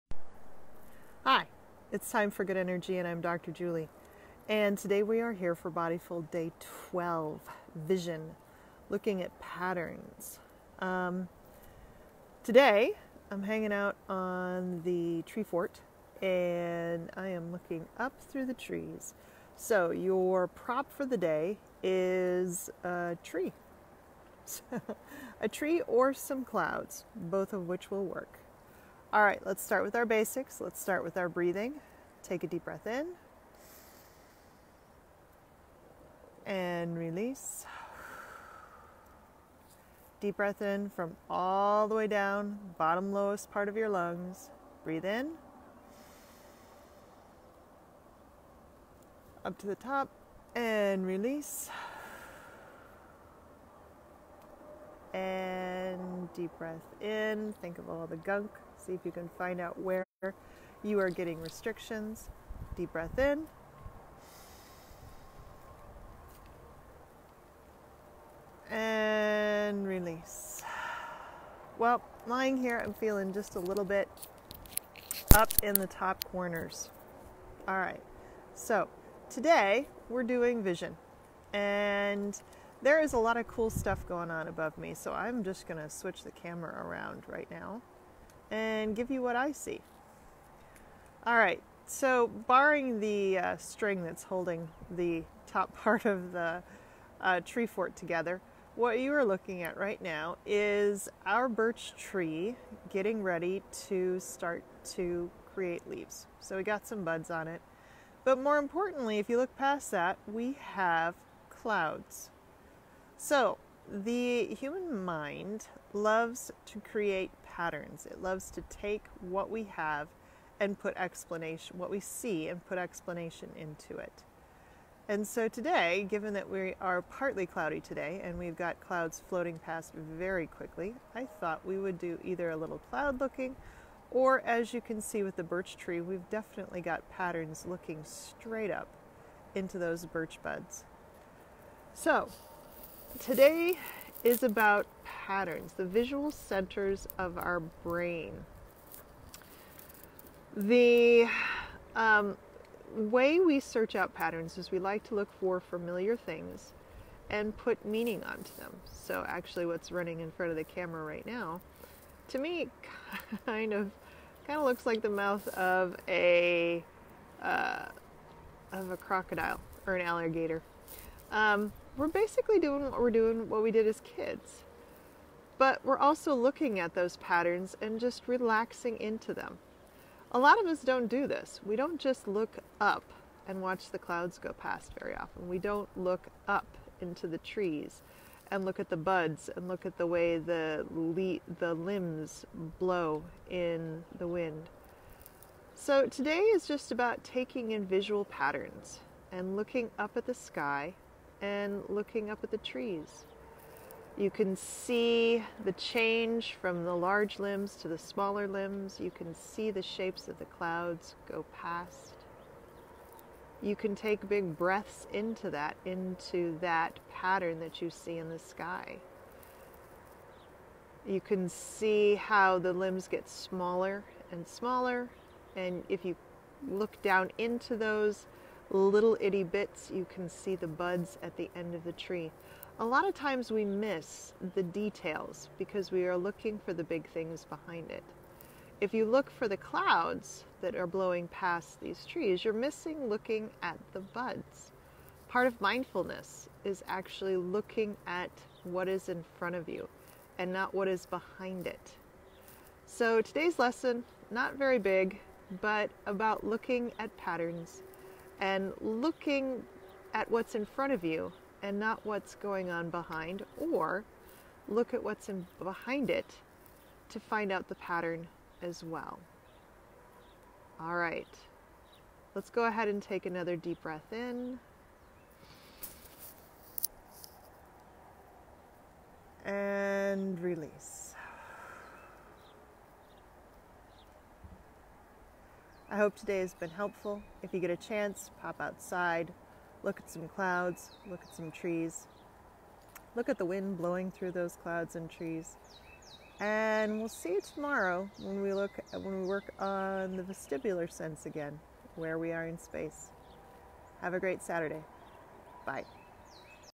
Bodyful Day 12: Vision - Looking for Patterns
Today we look up to the sky to find patterns. The human visual centers are programmed to group similar things together and create meaning where it can. Join me while we look at fractals in the trees and patterns in the clouds.